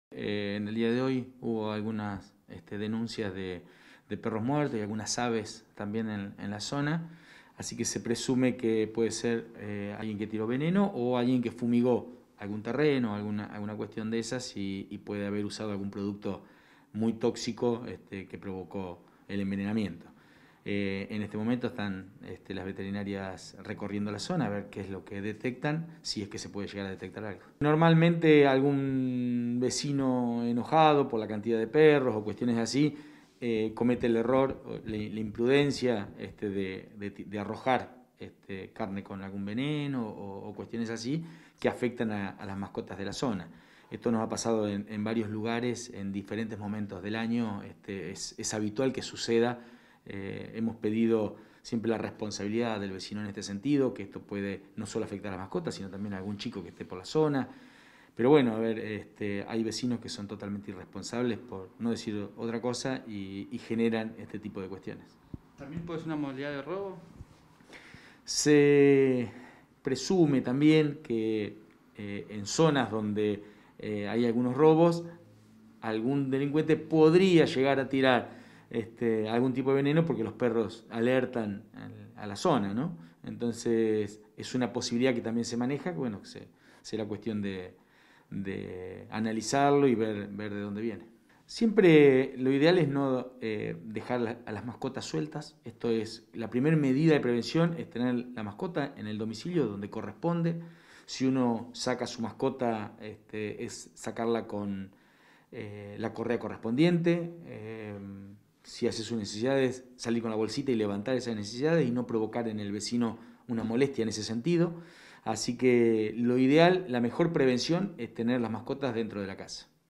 Vecinos denuncian envenenamiento de perros en Trelew
Ocurrió en barrio San David el sábado y este lunes se realizó la denuncia formal a la Municipalidad. Además de perros también se encontraron aves muertas. Se presume que puede ser alguien que tiro veneno o que fumigó con algún producto muy tóxico. Hablamos con el coordinador de inspecciones generales de Trelew al respecto.